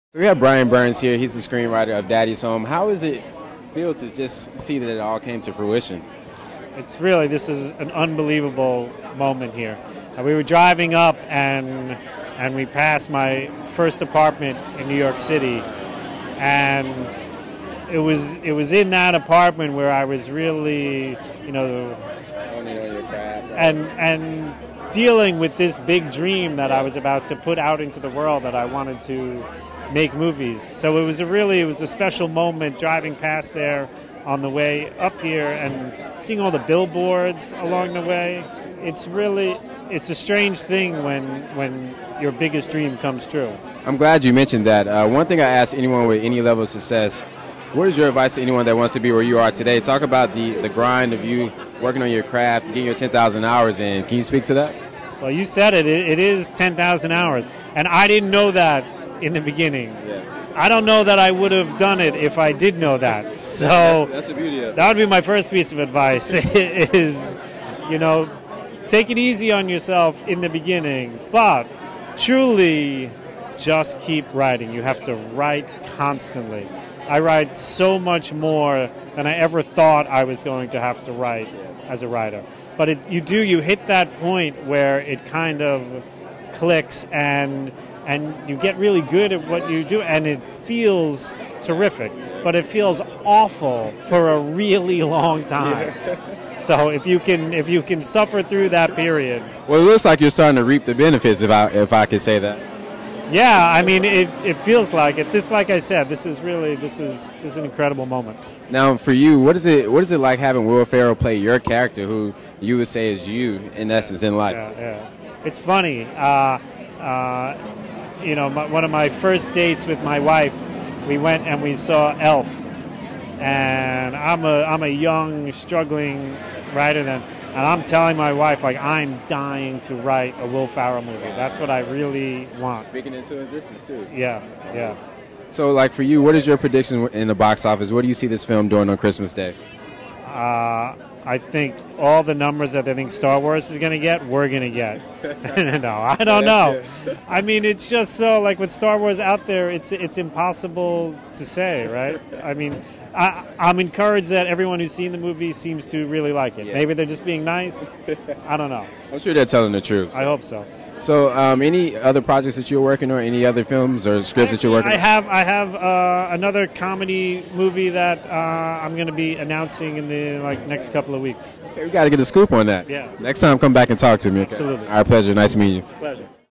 Screenwriter Brian Burns Talks Will Ferrell Portraying His Life | Daddy's Home NYC Premiere
Television host Kamaron Leach talks with Brian Burns, the screen writer of Daddy's Home, about putting in your 10,000 hours, Will Ferrell portraying his real life character, and accomplishing your goals. Brian also has writing and producer credits for the well known television series 'Entourage'.